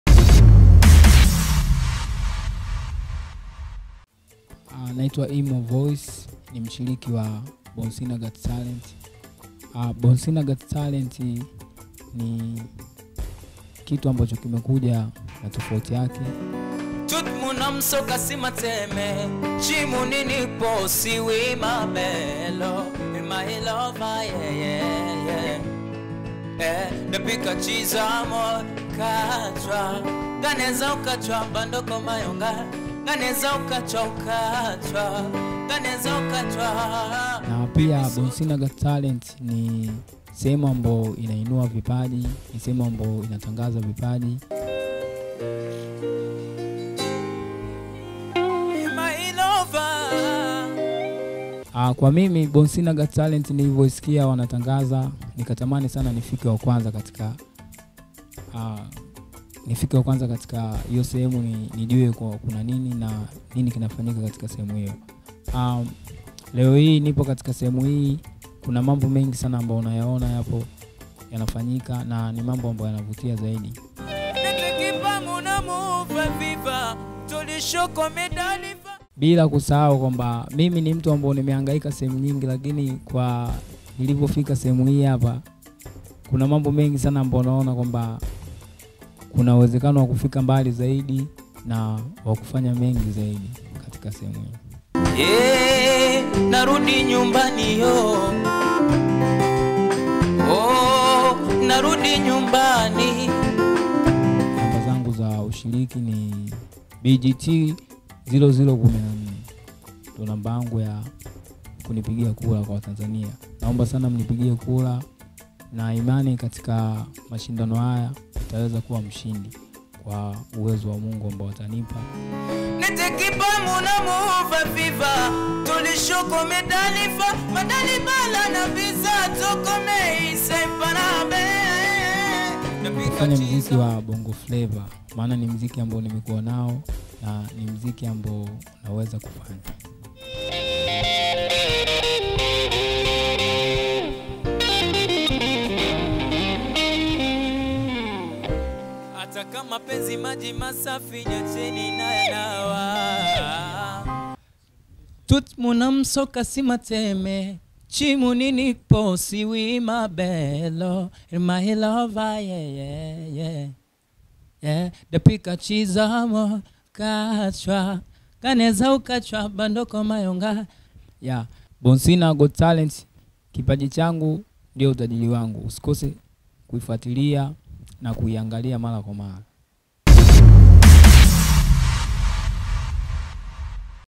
Boncena got talent2020/IMO VOICE mshiriki anaetamba 2020
Boncena got talent2020/IMO VOICE mshiriki anaetamba 2020|mwenye BGT-0014\akiwa anafanya MAZOWEZI ya sauti na vyombo mpezapot kijana wa Tanzania and iPad charger ajabu sana#boncenagottalent#Americagotalent